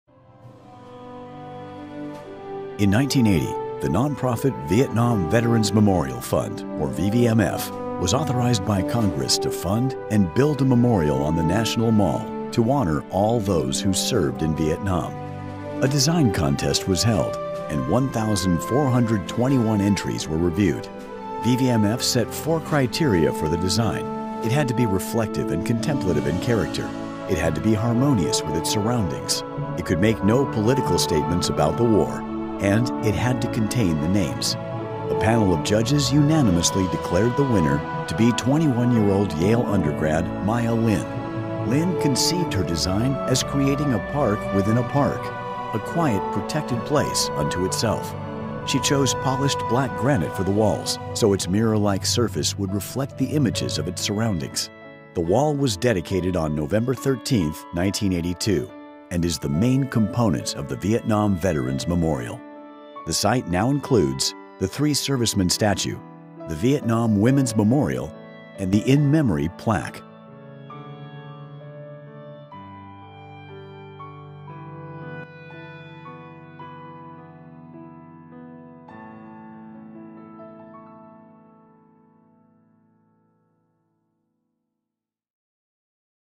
2023 History of the Vietnam Veterans Memorial
Learn more about the history and effort that went into getting the Vietnam Veterans Memorial built in Washington, D.C. A design competition was held and the winner was 21-year-old Yale undergrad Maya Lin.